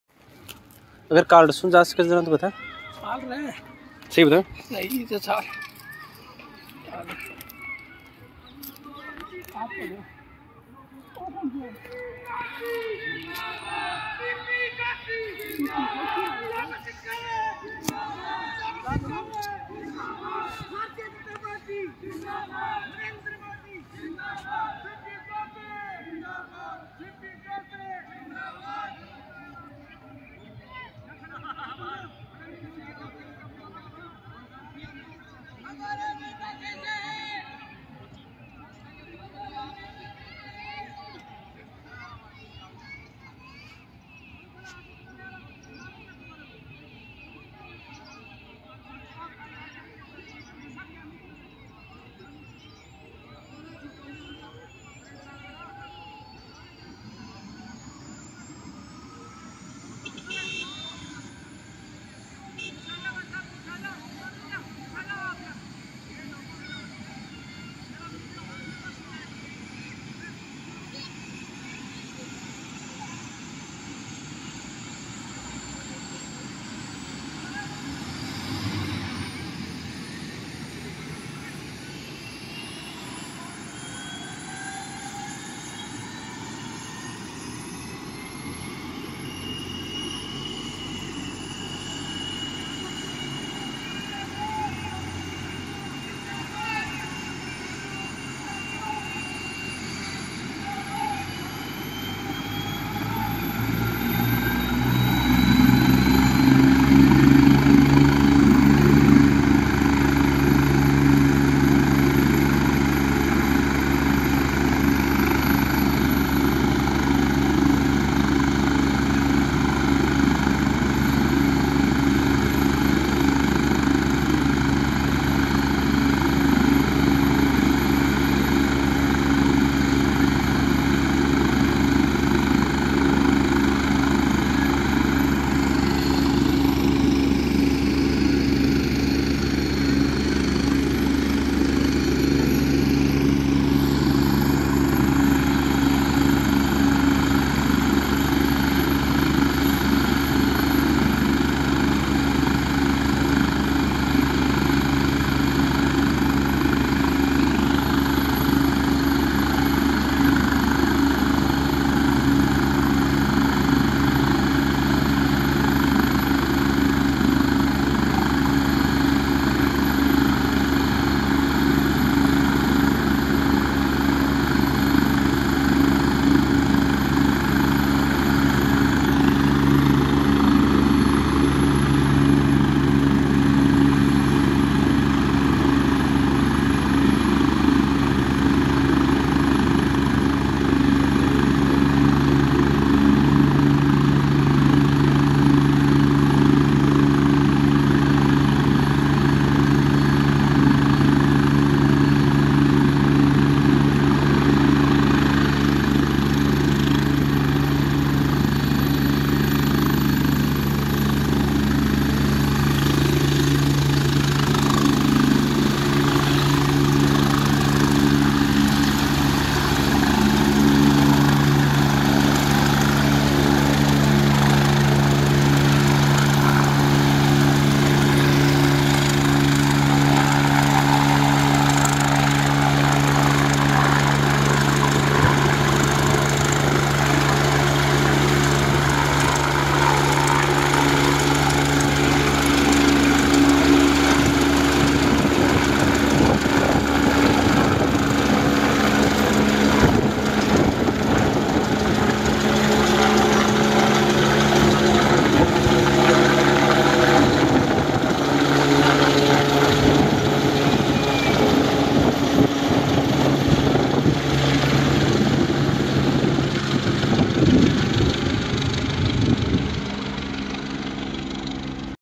श्रीमाधोपुर में सीपी जोशी का आगमन || झाबर सिंह खर्रा के समर्थन में सीपी जोशी की सभा
Video from HR NEWS 24